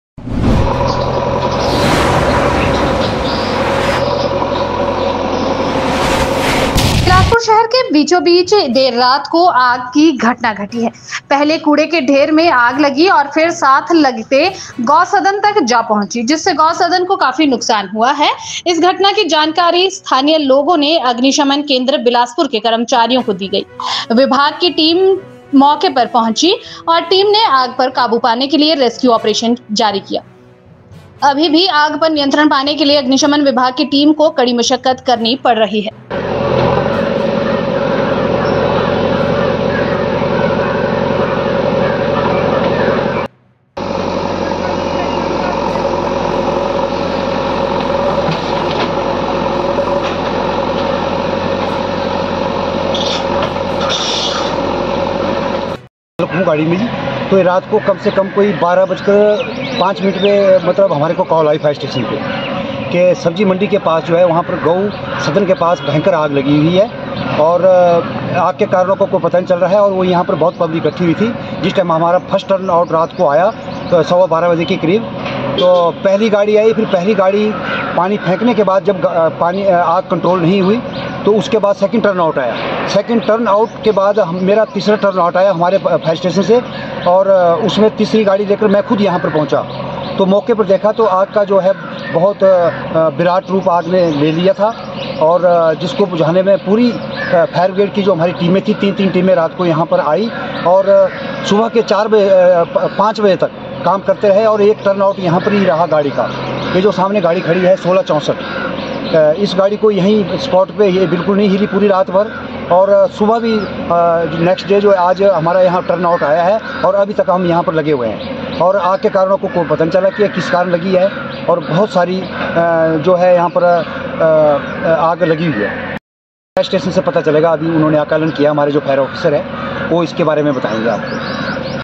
Bilaspur | Garbage fire | Cow House
A fire incident occurred late at night in the heart of Himachal Bilaspur city. First the fire broke out in the garbage heap and then spread to the adjacent cow shed. Due to which Gau Sadan has suffered a lot. Local people informed the employees of fire station Bilaspur about this incident. The department team reached the spot and launched a rescue operation to control the fire. The fire department team is still having to work hard to control the fire.